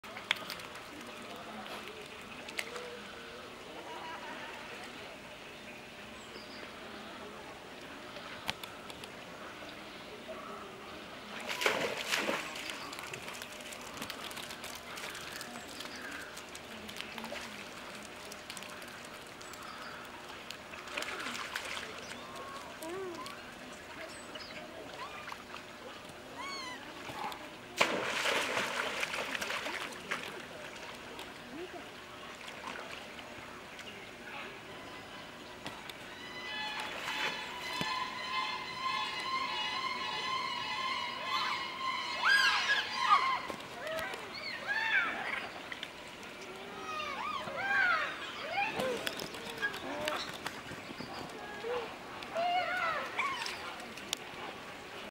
Japanese Macaque of children playing in the water (diving).ニホンザルの子供たちの水遊び（潜水）。
Video second half, noted that the monkey is in full swim underwater! !
ビデオ後半、猿が完全に水面下を泳いでいる事に注目！！
Tama Zoological Park.多摩動物公園。